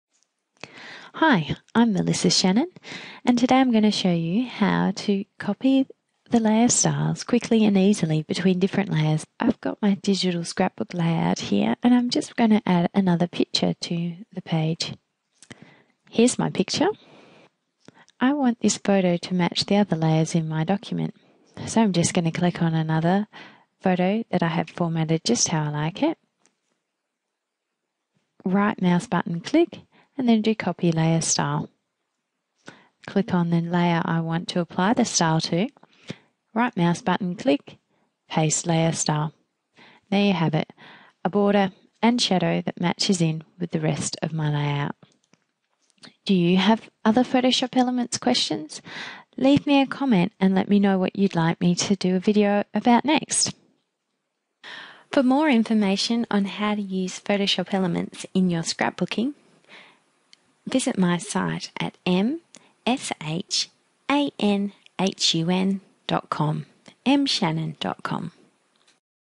Copy layer styles in Photoshop Elements
Copy the layer styles from one layer to another with this quick tip in Photoshop Elements 8

Copy style settings between layers

In the Layers panel, select the layer containing the style settings you want to copy.
Right click on the layer name and click Copy Layer Style.
Select the layer you want to apply the styles to in the Layers panel, and right mouse button click on the name and select Paste Layer Style.
copy layer styles

Supplies:

Jen Lindsay's templates from The Digi Game

Freebie from Flergs -- Dreaming of Spring via her newsletter The The kit this freebie coordinates is Dreaming of Spring

Dreaming of Spring: THE KIT

Let me know what else you'd like to learn about Photoshop Elements in the comments.